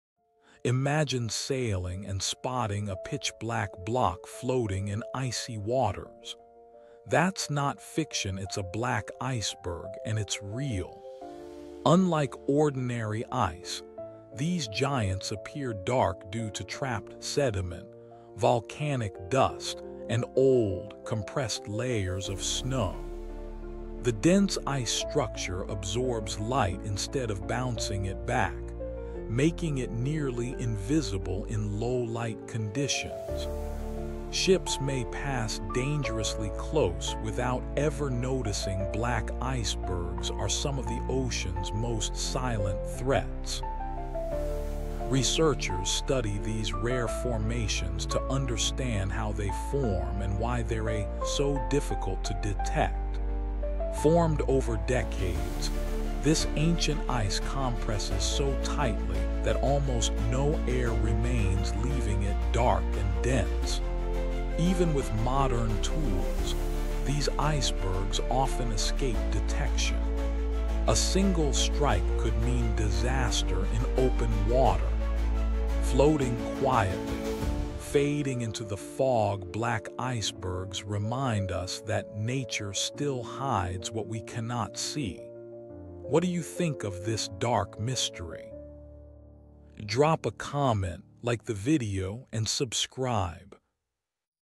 ⚫ The Black Iceberg That Defies Nature!
Most icebergs are white. But what if one appeared… completely black?

This rare natural phenomenon is called a black iceberg — formed by old ice, compacted snow, and volcanic or organic particles trapped inside.

Unlike regular icebergs, these absorb light instead of reflecting it, making them nearly invisible in the ocean, especially at night or in stormy weather.

In this short documentary, you'll discover:

How black icebergs are created

Why they’re extremely dangerous

And what scientists have uncovered about them

🌊 Still think all icebergs are harmless? Think again.

🧊 Watch now and be amazed by nature’s hidden wonders.